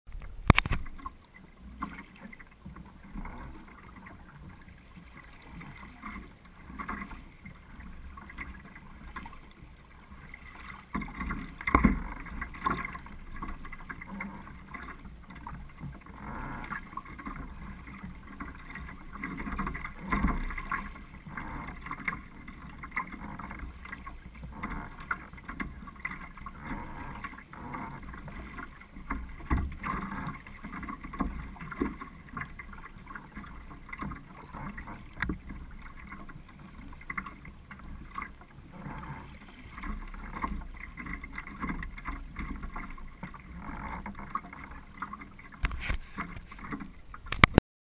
Tour en Corse en pirogue. Les dauphins s'invitent dans la course